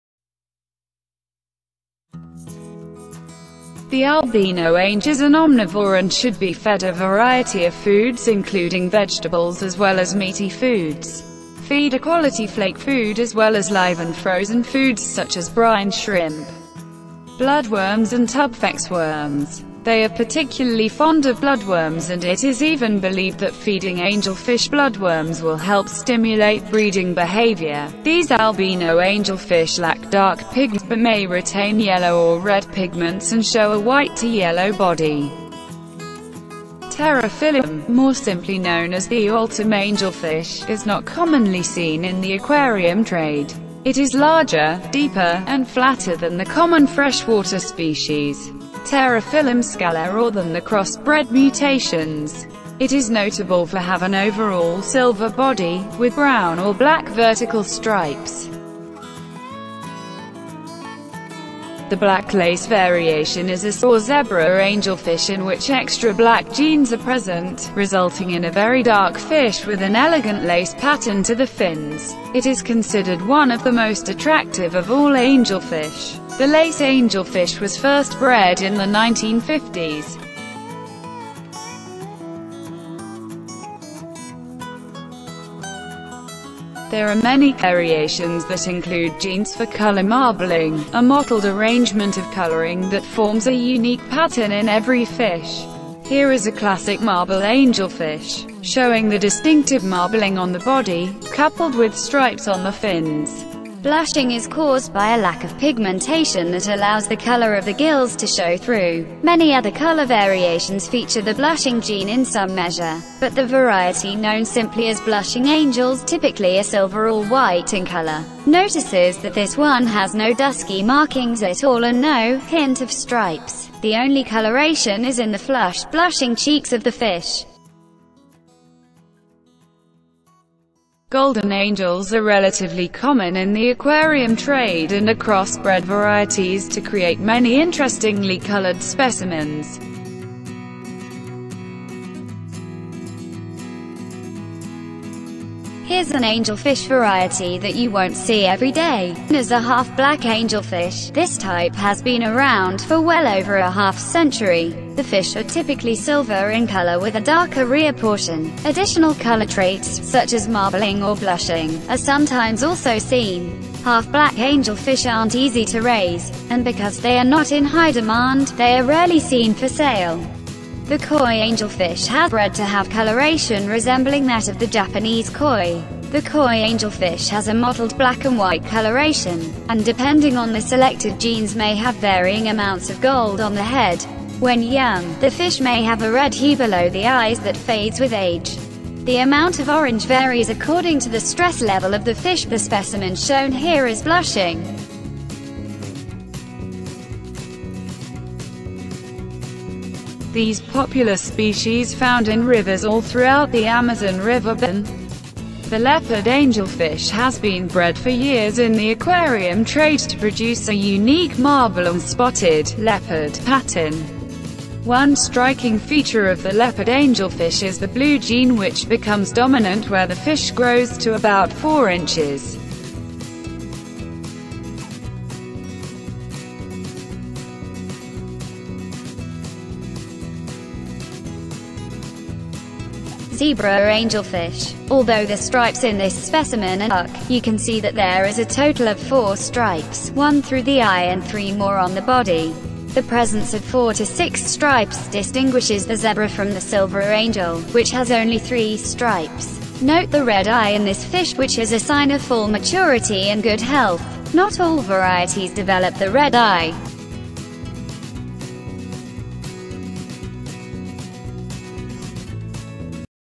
The Varieties of Angelfish Angel Fish Aquarium
Freshwater angelfish comprise three different native species in the Pterophyllum genus: P. scalare, P. angelfish, and P. altum. Those sold as domestic aquarium pets are usually varieties of the P. scalare species, although they can be somewhat confusing to identify genetically, as the species cross-breed easily. Some varieties may be crosses between P. scalare and P. altum.

Domestic freshwater angelfish are the result of many years of selective breeding of various color attributes, such as background body color, striping, spotting, "blushing,"  marbling (orange and black). The aquarium trade offers many different color variations, although some may be difficult to find. planted tank aquatics how to keep tropical fish